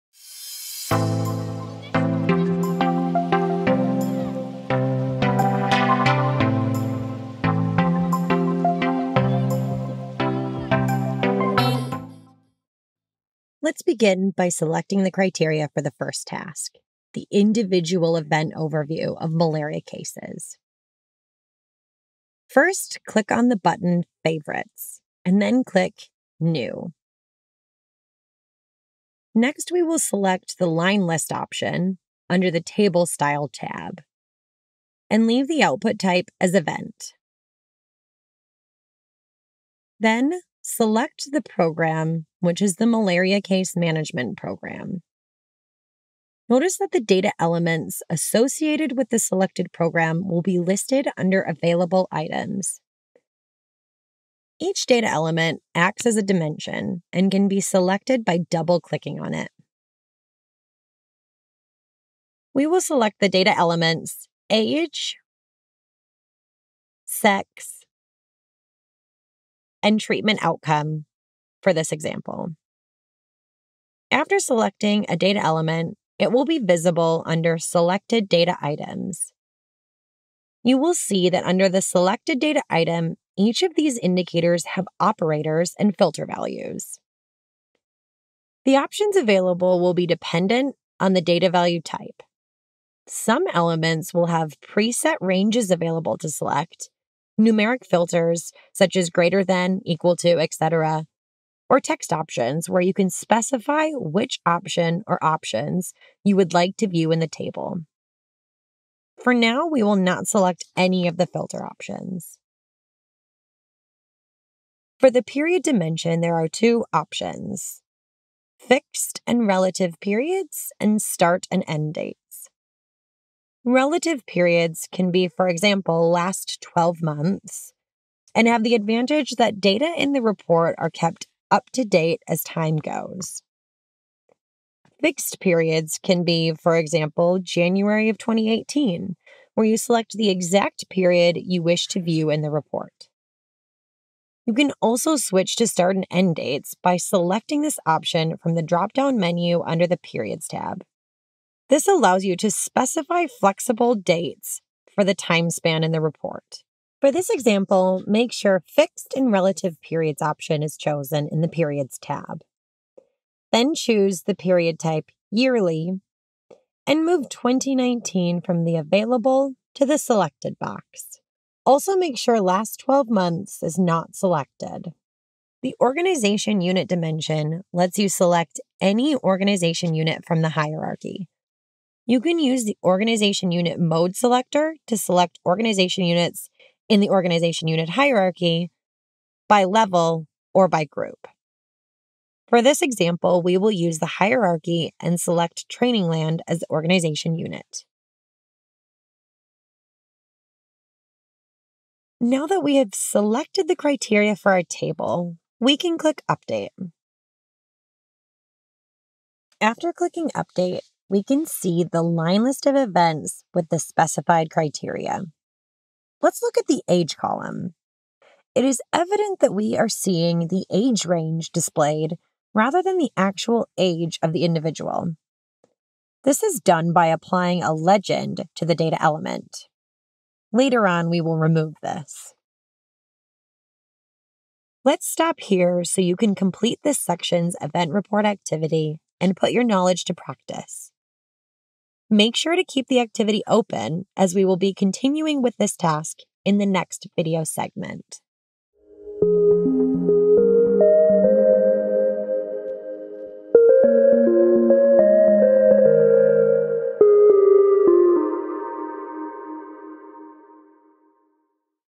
Module 4: Subsection 1 - Event Reports Demonstration [Part 2 of 6]
[Part 2 of 6] This demonstration gives an overview of the Event Reports app in dhis2. Key features are described in detail, and a demonstration is given on how to view data using these reports.